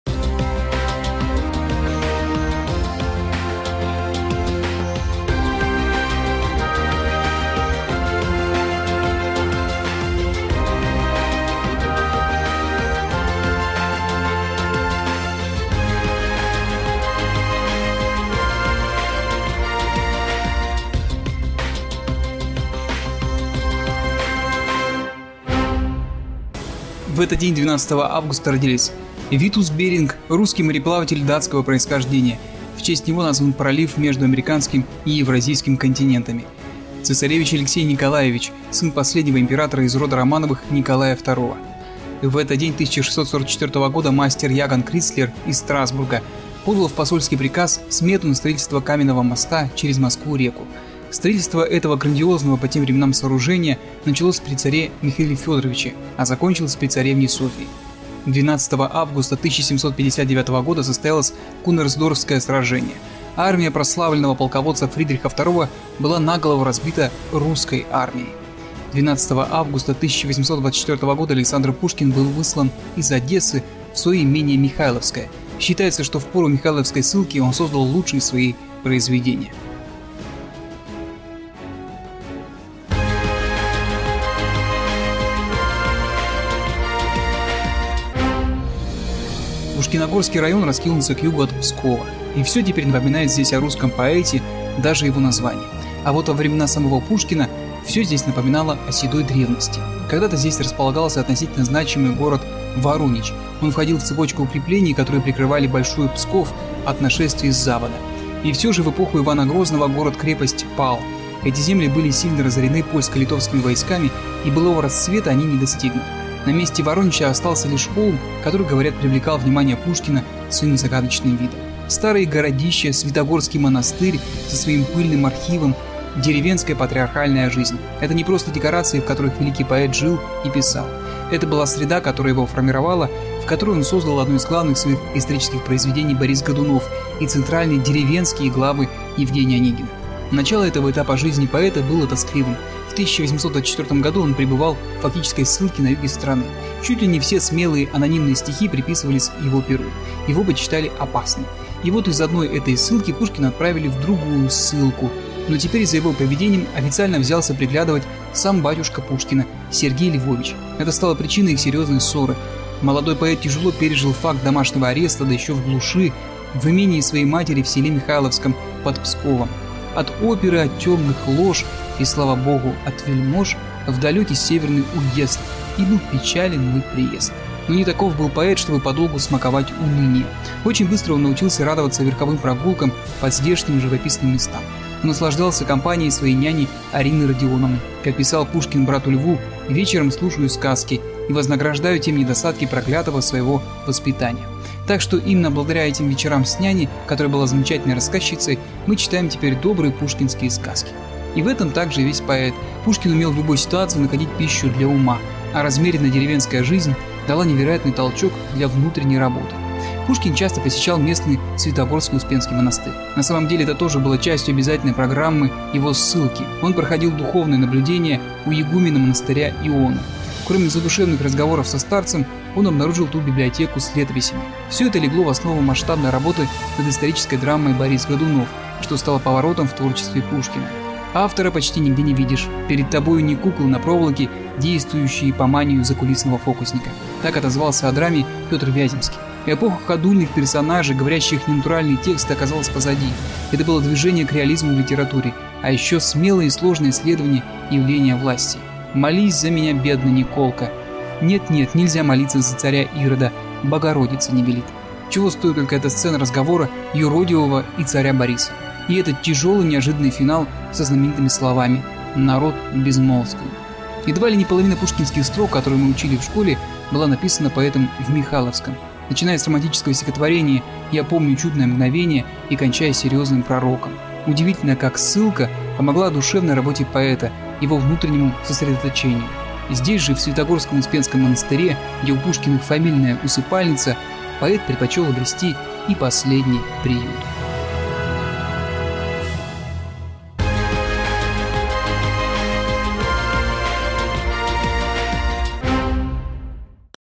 Этот день в истории. 12 августа 2016
12 августа 1824 года Александр Пушкин был выслан из Одессы в свое имение Михайловское. Считается, что в пору михайловской ссылки он создал лучшие свои произведения.